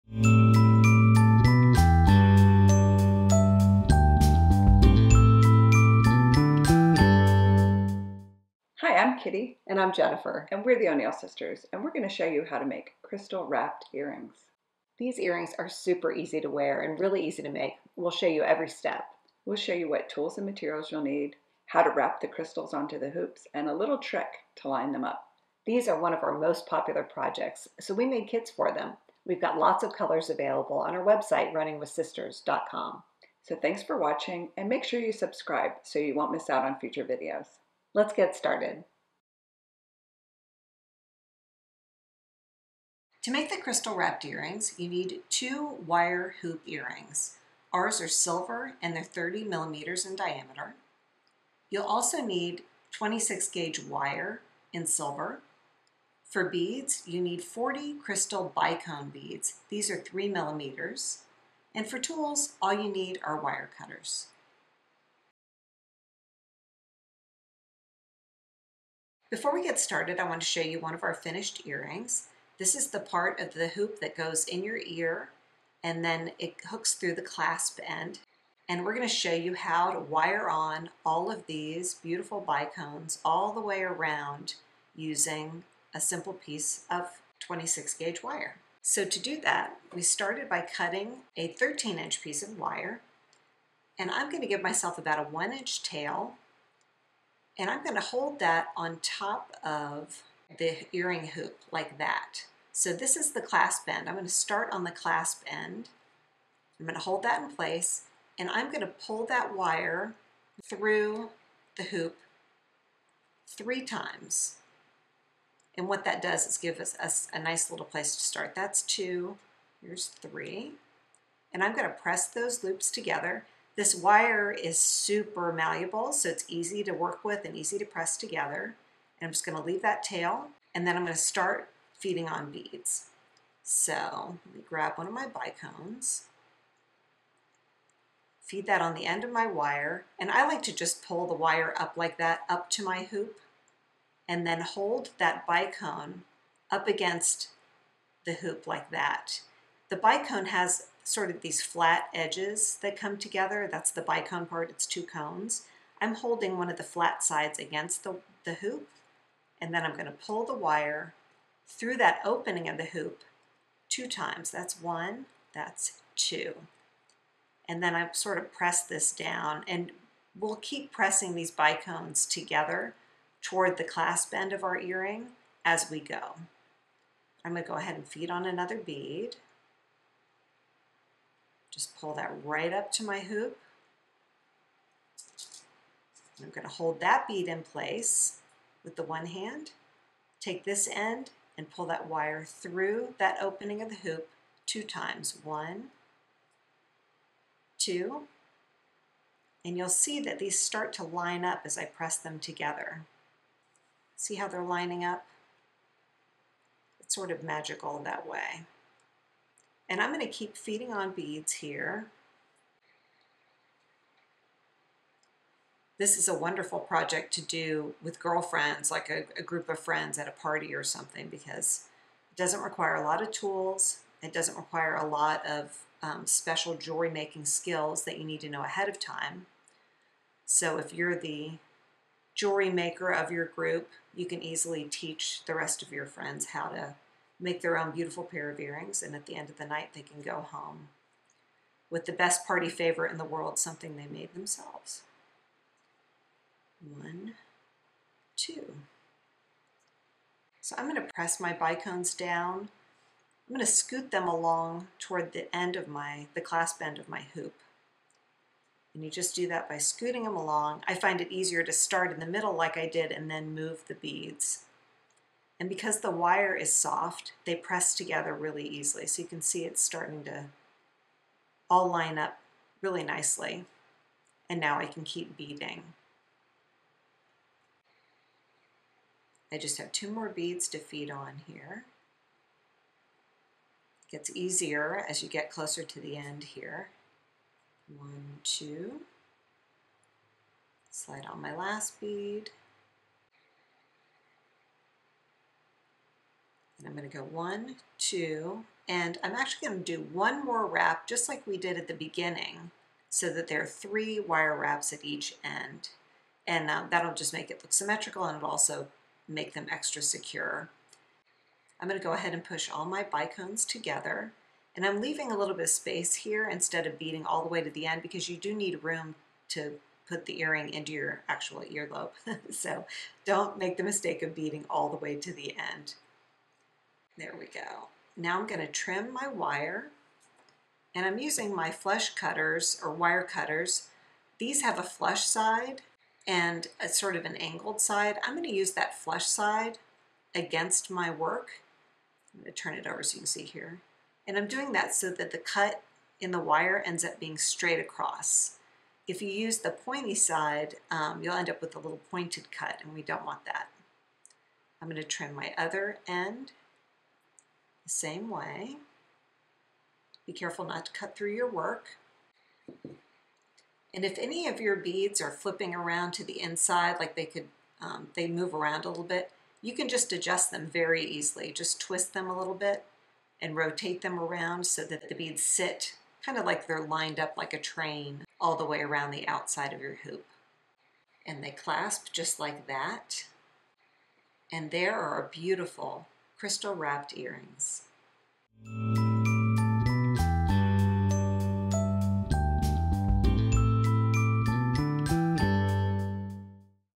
Delicate Crystal Wrapped Earrings
In “Delicate Crystal Wrapped Earrings" we’ll show you how to turn a pair of plain silver hoops into dazzling but delicate Crystal Wrapped Earrings! All you need is the right gauge wire and beautiful bicone crystal beads. In this jewelry video tutorial, we’ll show you how to wrap the hoops with wire, adding crystal beads as you go, and we’ll share some special tips and tricks along the way. This is one of our favorite projects to do with friends!

Let’s connect!

Chapters:
0:00 Introduction
0:39 Tools & Materials
1:06 Wrapping The Hoop
2:12 Adding The Beads
4:56 Finishing The Beading
5:29 Trimming The Ends
6:24 Finished Earrings

How To Make Delicate Crystal Wrapped Earrings
by Jennifer and Kitty O'Neil
Running With Sisters

Learn how to turn a pair of plain silver hoops into dazzling but delicate Crystal Wrapped Earrings! All you need is the right gauge wire and beautiful bicone crystal beads. The rest is easy! We'll show you every step below, or watch the video we made to see the earrings being made in action!

Materials & Tools
40 Crystal bicone beads, Black Luster, 3mm
2 Hoop earrings, Silver, 30mm
2 Lengths 26-gauge silver wire, 13" each
Wire cutters

Steps
1. Start by holding one end of the 13" wire against the hoop leaving a 1" tail.
2. Wrap the wire around the hoop three times. Press the wraps together and slide them to the beginning of the hoop.
3. Slide a crystal bicone bead onto the wire and down to the earring hoop.
4. Hold the bead in place against the hoop and wrap the wire around the hoop two times.
5. Repeat to add beads, wrapping the wire two times between each bead. Push the wraps and beads together as you work.
6. Continue until you have 20 crystal beads wrapped onto the hoop.
7. After the last bead, wrap the wire around the hoop three times.
8. Use wire cutters to trim both ends of the wire close to the hoop.
9. Repeat to bead the second hoop earring.

Aren't these delicate Crystal Wrapped Earrings pretty?! We'd love to hear from you. Do you think you'll make a pair?
Happy Crafting,
Jennifer & Kitty

If you like this video, you may also like these jewelry making how-to videos: